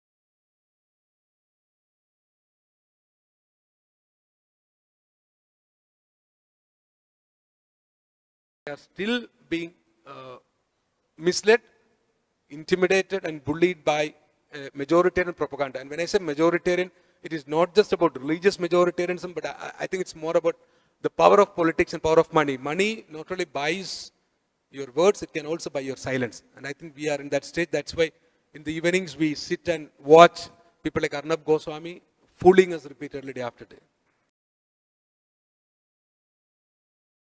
Josy Joseph on Arnab Goswami
Josy Joseph,author of 'A feat of vultures', the encyclopedia of corruption in India, describes Arnab Goswami brand of 'news'